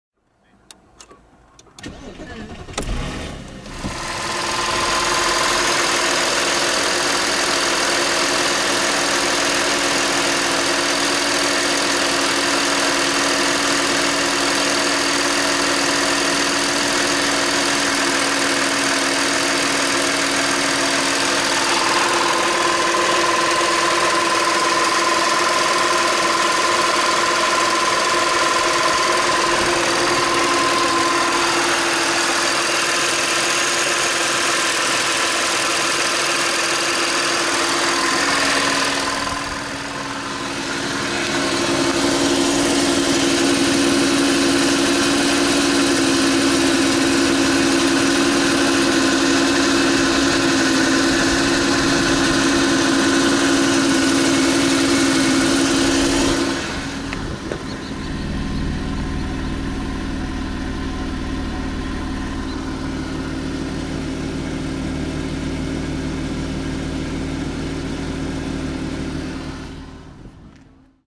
Audi A6 C6 (4FH) V6 3.2FSI 188kW, original timing chains sound
Audi A6 C6 Allroad (4FH) V6 3.2FSI 188kW

295 000 km with original timing chains.
Sound before maintenance and exchanging of some parts.